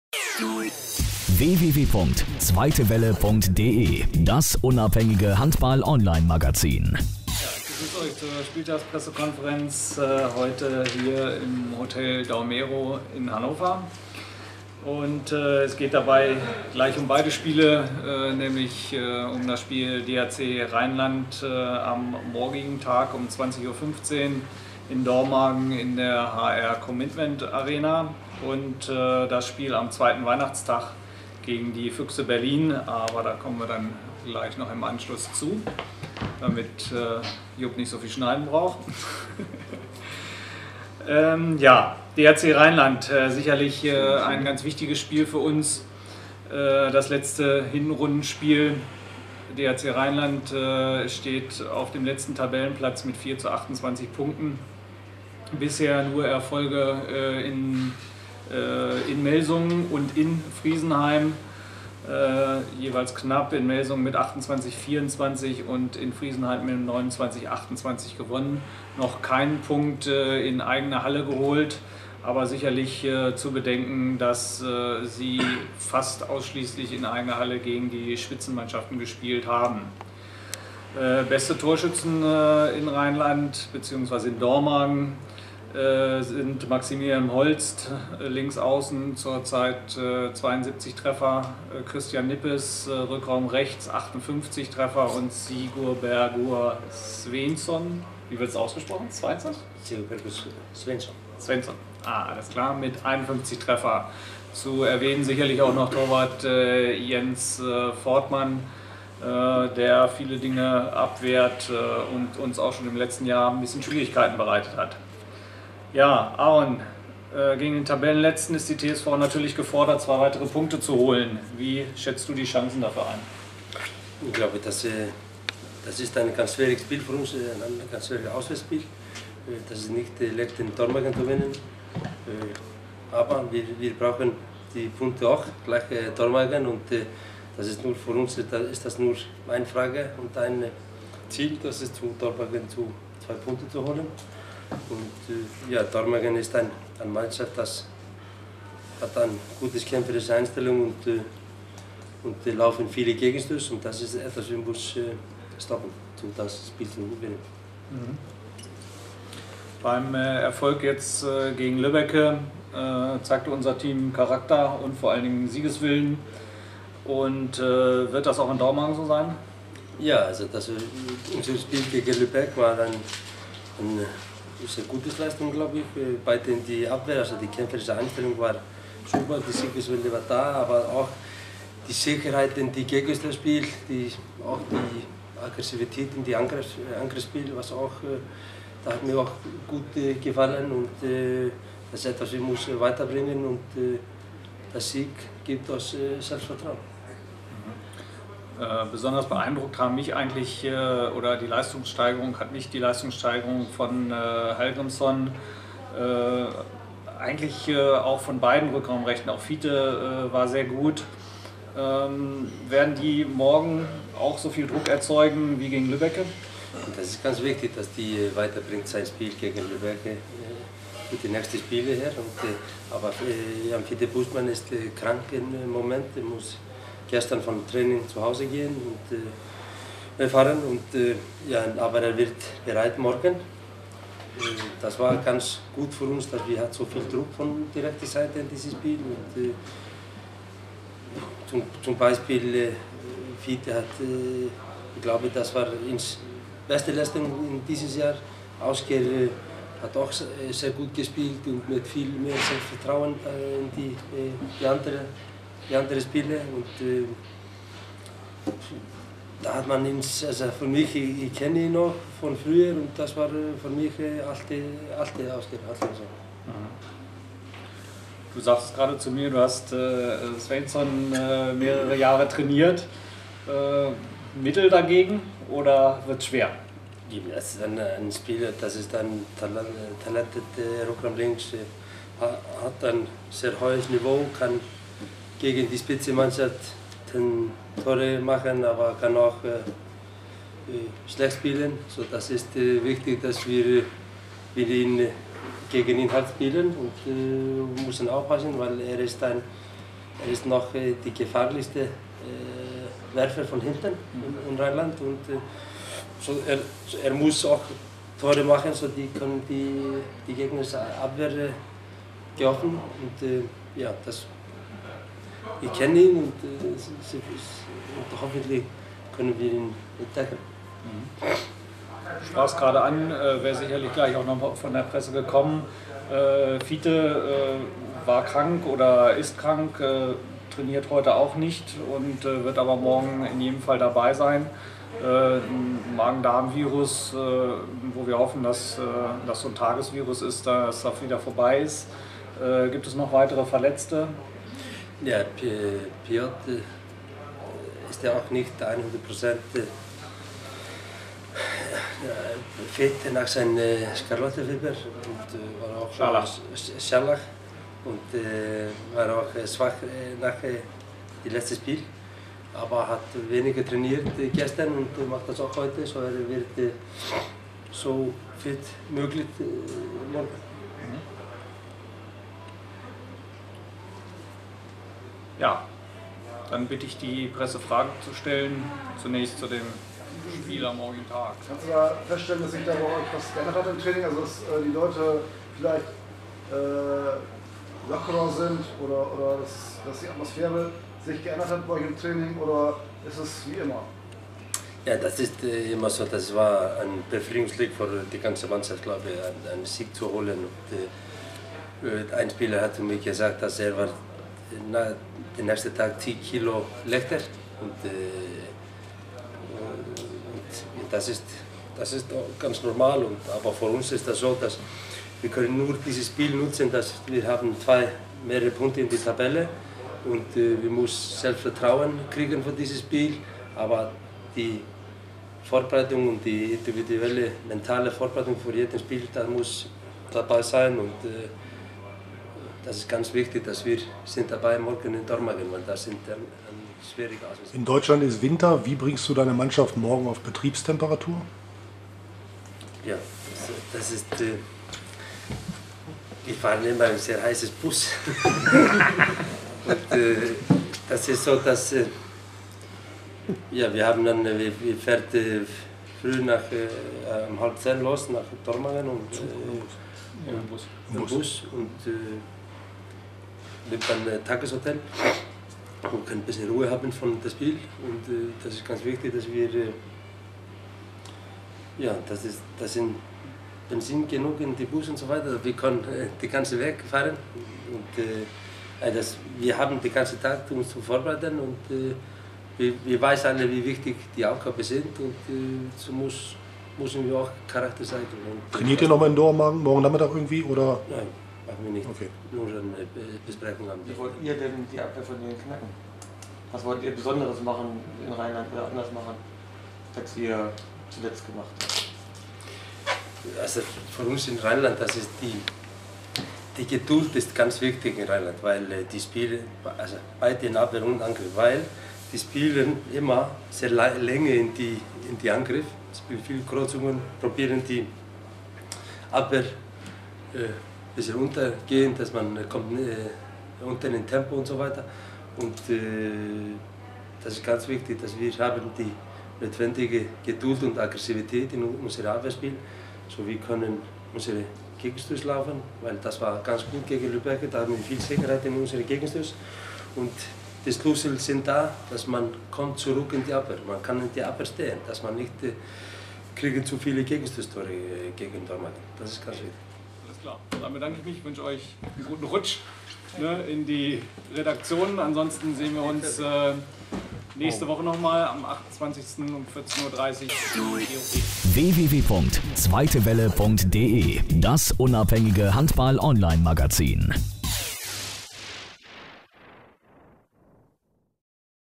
PK DHC Rheinland vs. TSV Hannover-Burgdorf vom 21.12.2010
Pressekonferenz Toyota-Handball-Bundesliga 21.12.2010
DHC Rheinland vs. TSV Hannover-Burgdorf vor dem HBL Punktspiel am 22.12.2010 um 20.15 Uhr aus der HR-Commitment Arena in Dormagen
Hannover´s Trainer Aron Kristjansson und Pressesprecher Holger Staab